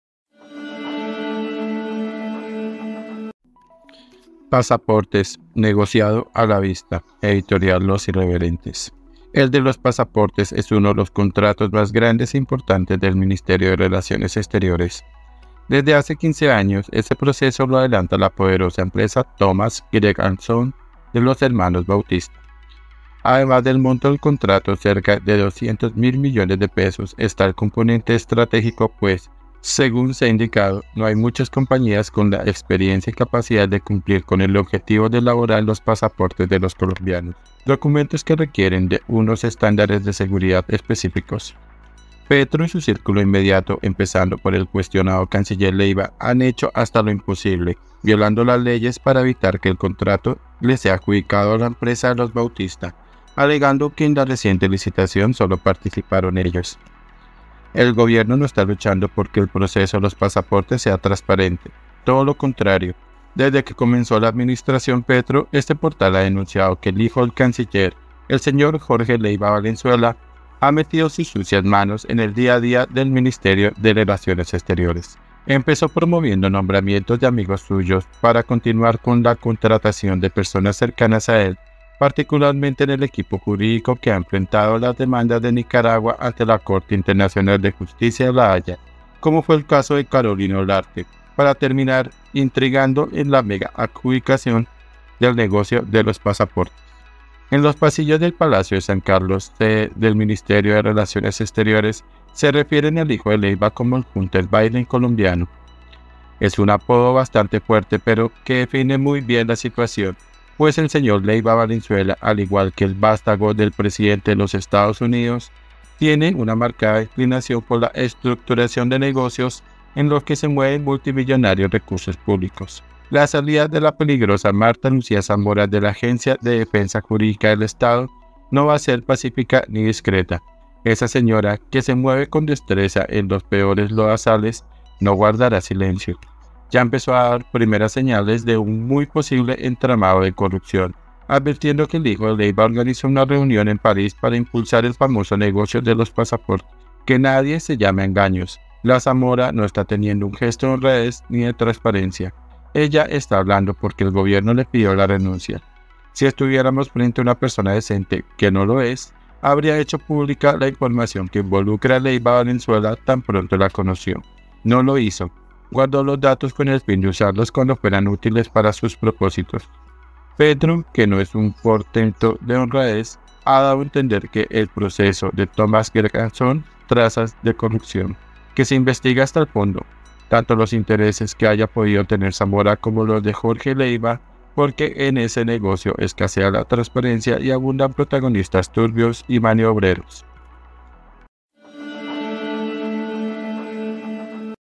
LOS NEGOCIOS DEL HIJO DE LEYVA || PASAPORTES: NEGOCIADO A LA VISTA ⛔ Editorial Los Irreverentes
El de los pasaportes es uno de los contratos más grandes e importantes del ministerio de Relaciones Exteriores. Desde hace quince años ese proceso lo adelanta la poderosa empresa Thomas Greg & Sons, de los hermanos Bautista.